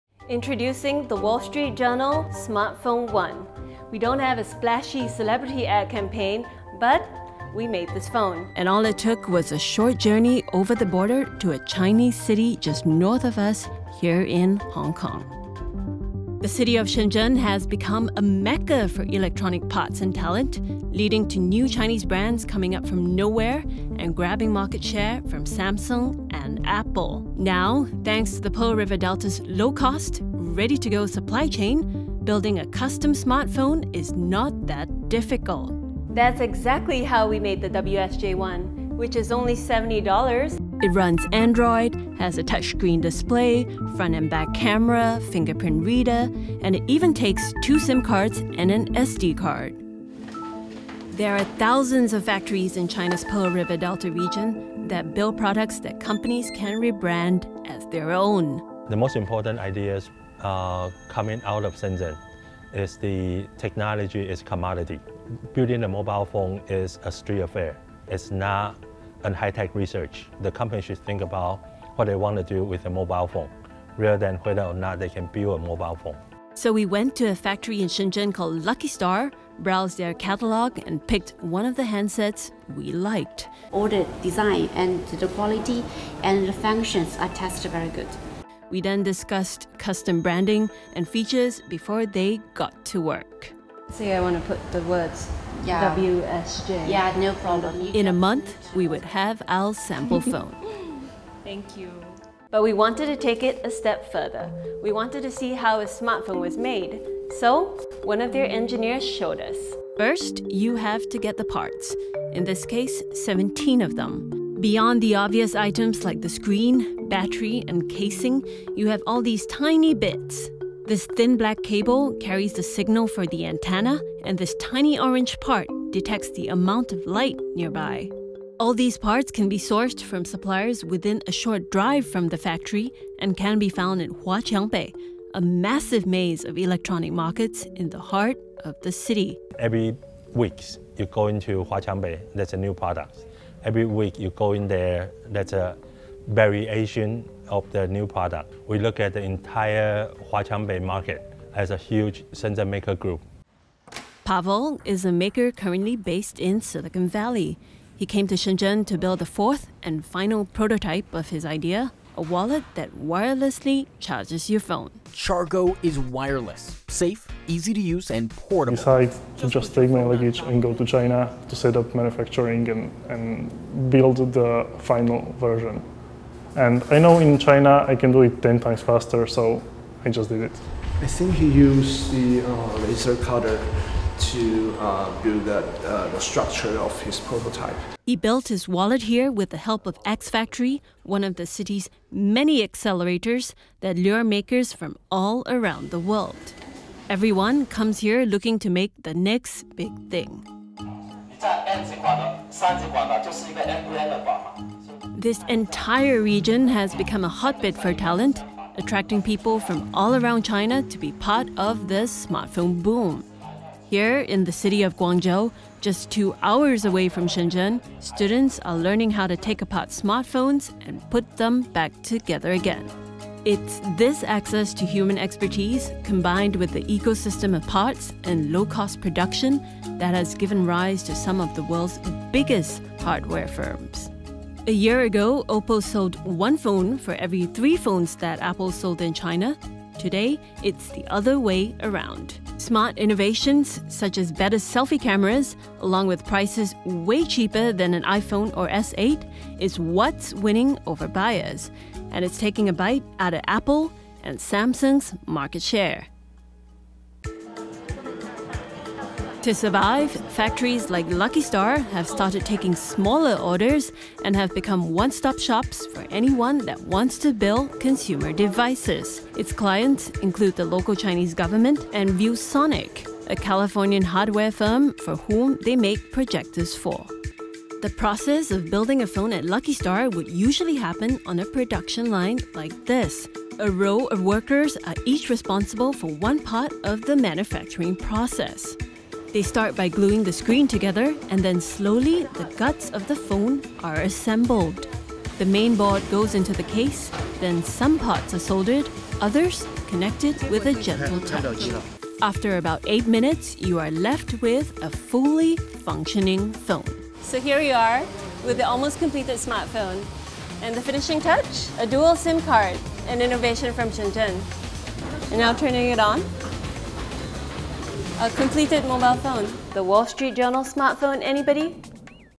How to Build Your Own Smartphone - for $70
Andy Rubin's phone cost $700; the WSJ 1 cost $70 and helps explain why the smartphone market is so tough. Take a journey to see how, with the help of Shenzhen's ready to go supply chain, we did it. Image: Thomas Di Fonzo